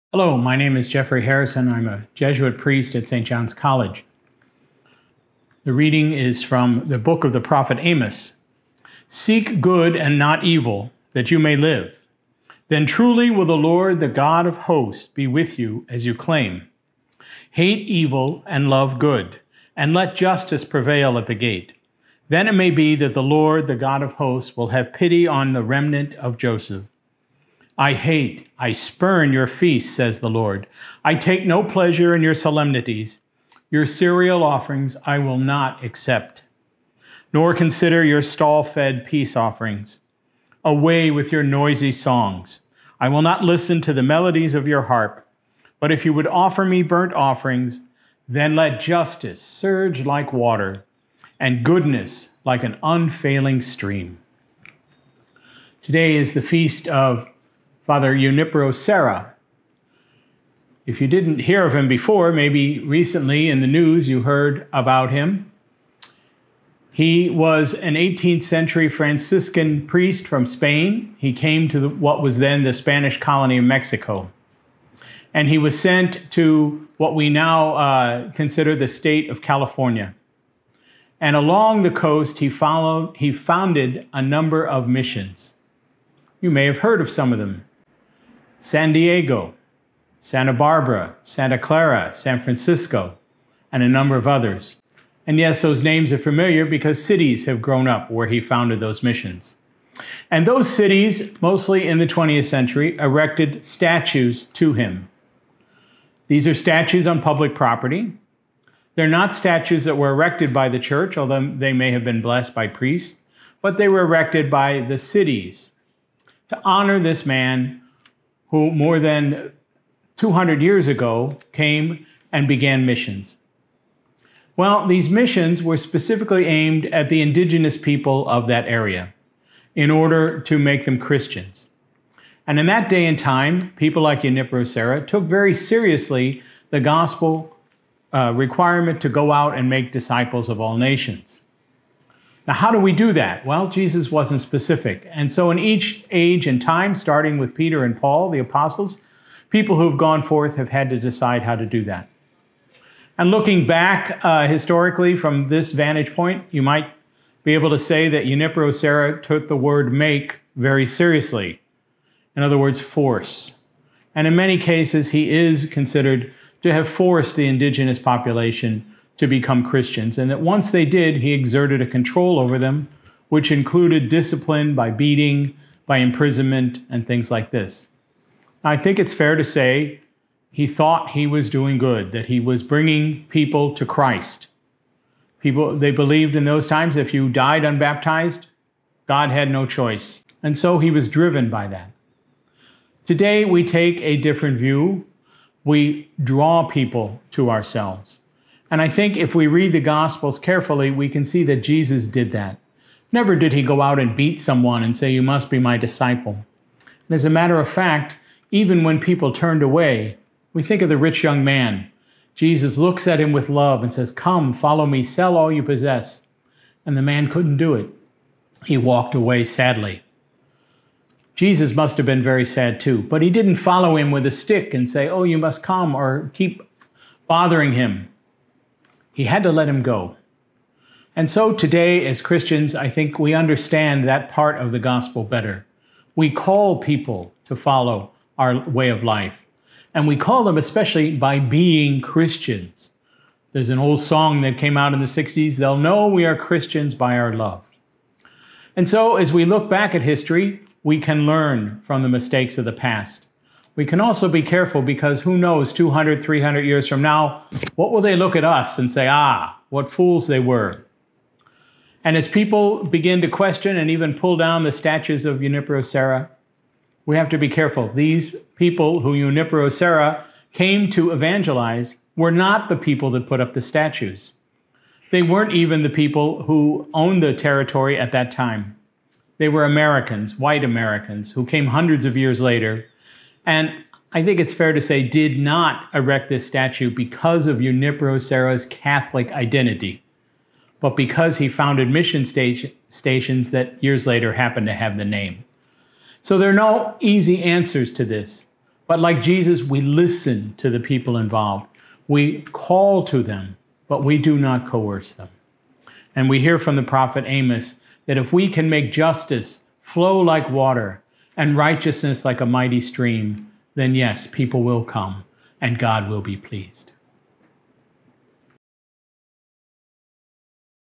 Daily reflection | July 1 (St. Junipero Serra)
Fr. Jeff reflections in the complicated legacy of today's saint, Juniper Serra.